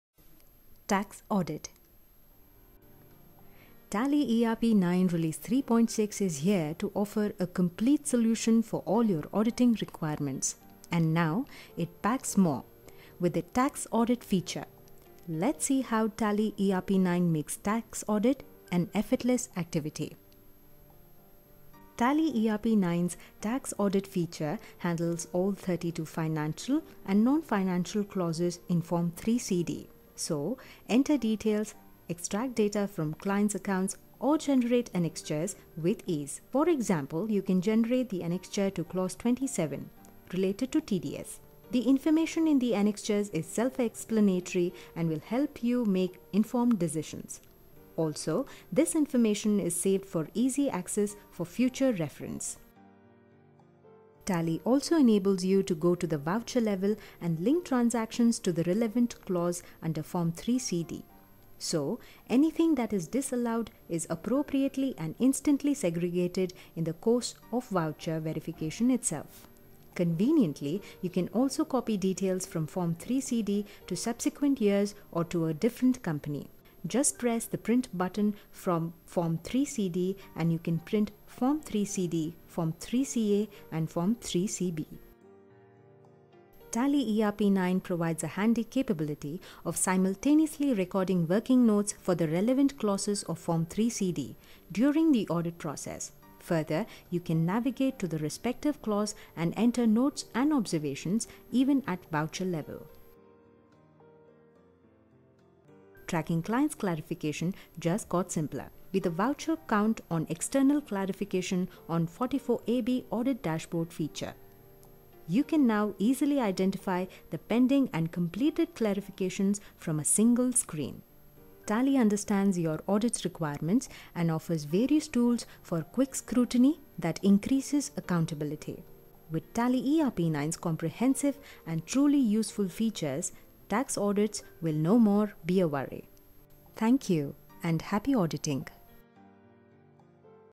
Tax Audit in Tally.ERP 9
Generate Form 3CA, 3CB, and 3CD with annexure of financial information
Map transactions to the relevant clauses of Form 3CD and its annexures
Create working note for the relevant clauses of Form 3CD
Option to mark single transaction to multiple clauses
Track client's clarifications with date and time